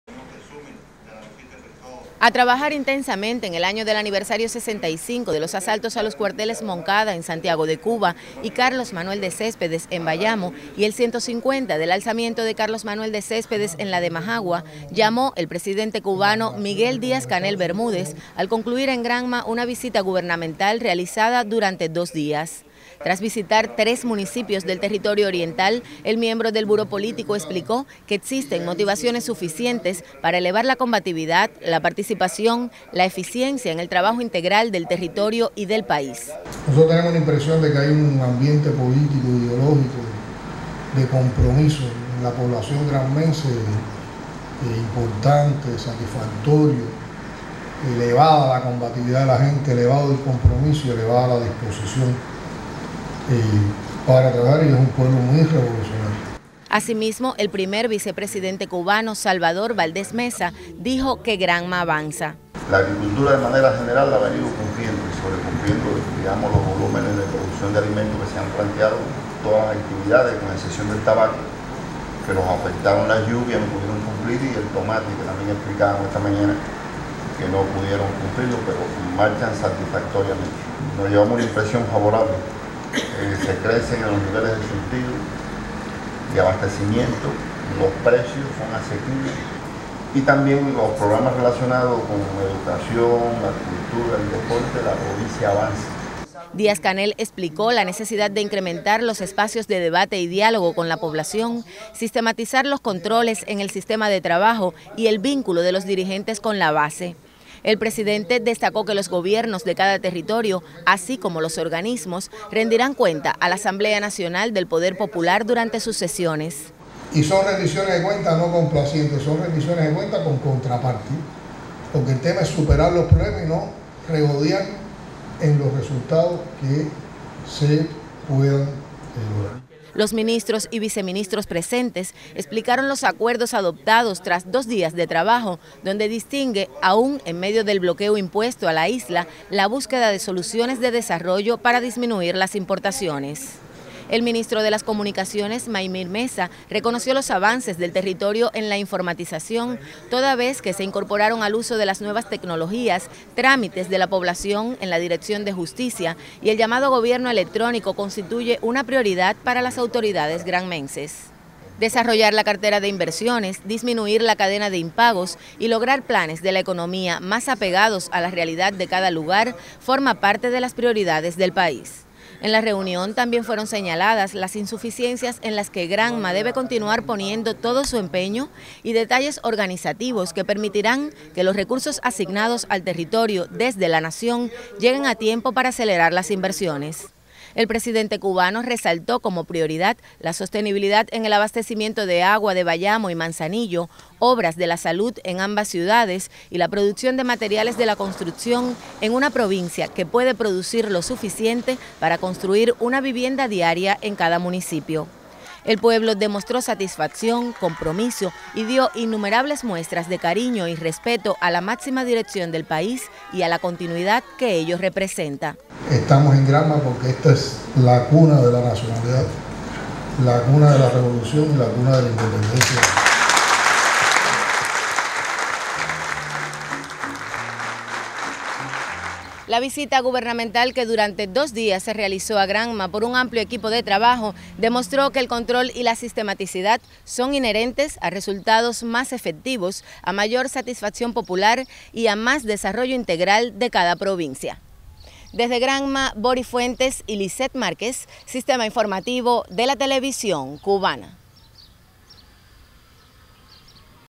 Preisdente Cubano concluyó su visita a Granma
Miguel Díaz-Canel Bermúdez finalizó su visita a Granma luego de dos días de intenso trabajo.